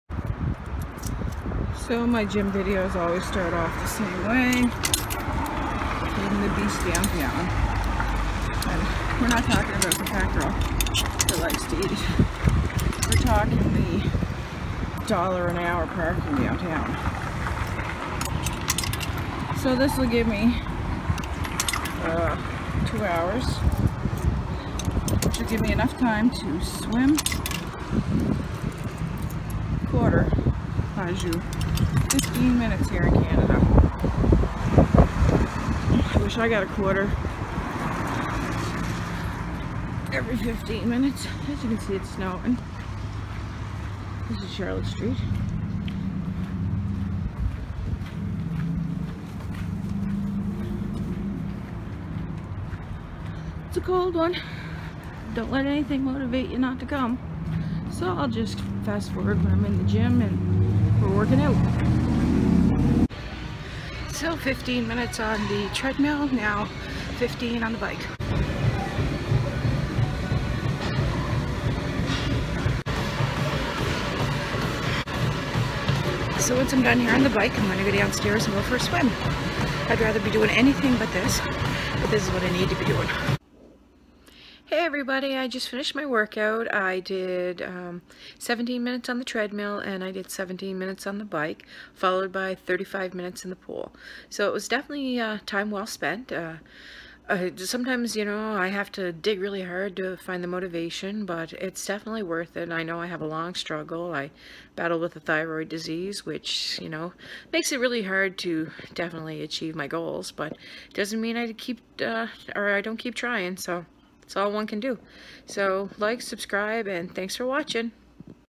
Working out at the Ymca Dec 20 2019
some of my viewers wanted me too do more updates on if I am hitting up the gym and working on my fitness its a hard struggle but one I keep trying